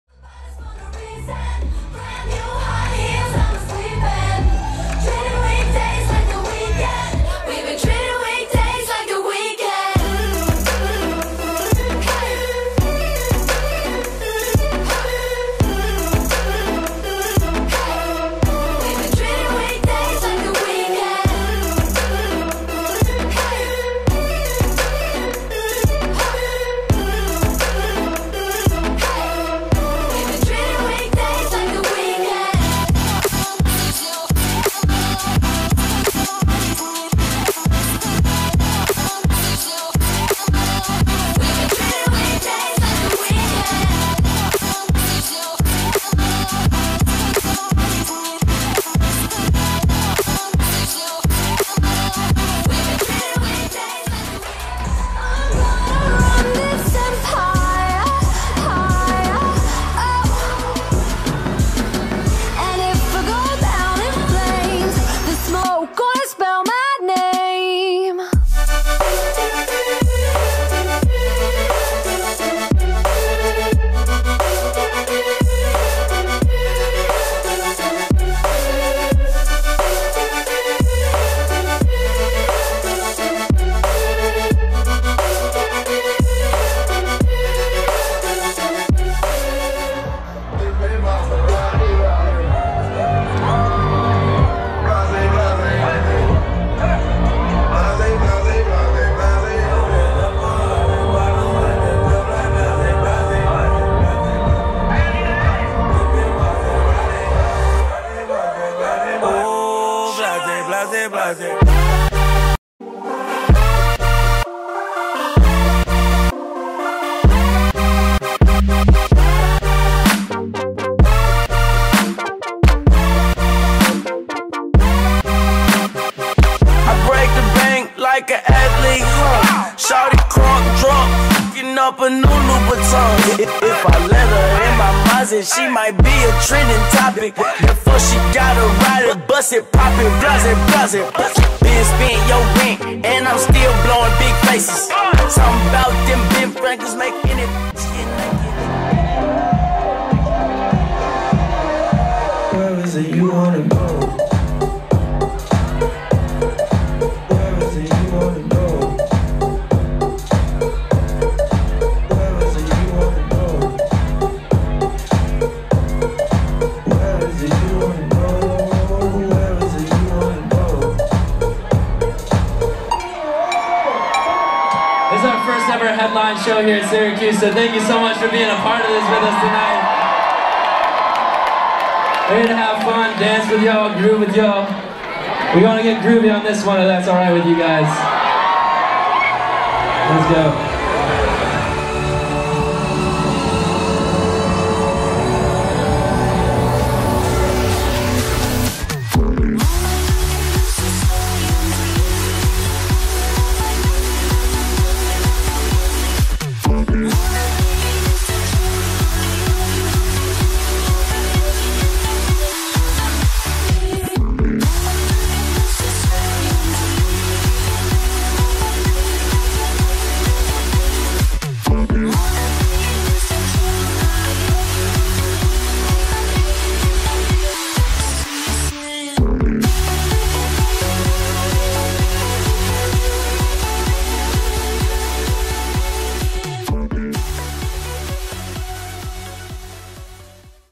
Louis The Child at Westcott Theater in 360
Watch Louis The Child play the Westcott Theatre at Syracuse University in 360!